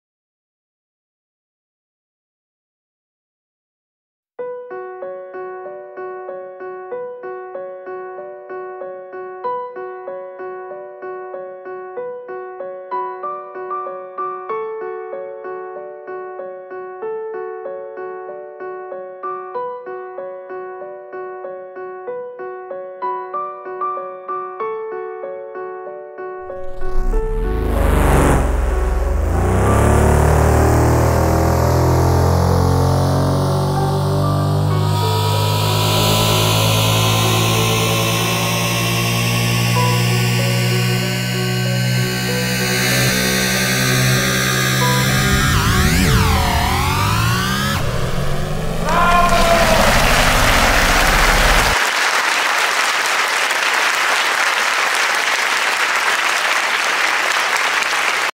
SNEAKY SKIDDERS
On a quiet sunday morning, for a stir, go and do some burnouts near the Impound yard, Surely they wont have any video cameras.